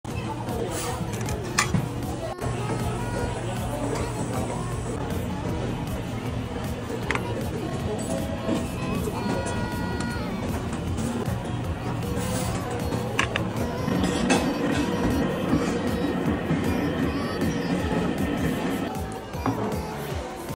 What I Ate at a Strawberry Buffet in Korea Part 2 🍓🇰🇷
What I Ate at a Strawberry Buffet in Korea // lemon cream strawberry tart, strawberry pana cotta, foret noir cake, strawberry cheese pop parfait, chocolate fondue